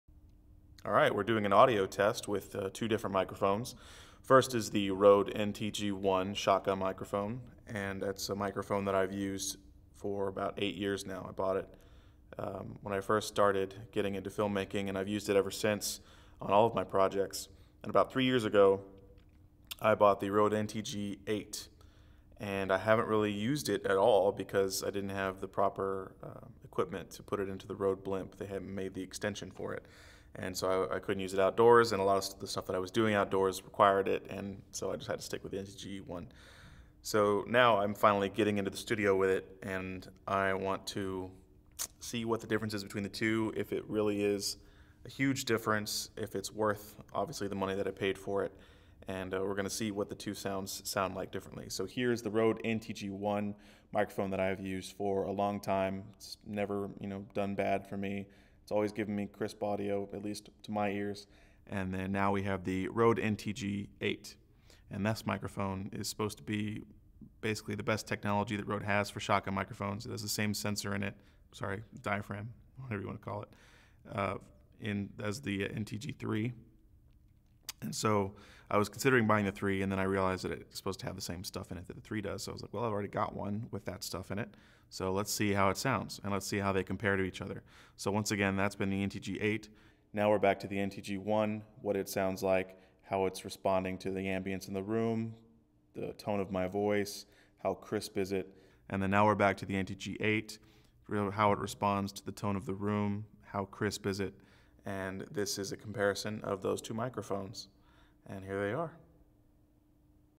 Rode Mic Test: NTG-1 Vs NTG-8
Testing the differences side by side of the Rode NTG-1 and Rode NTG-8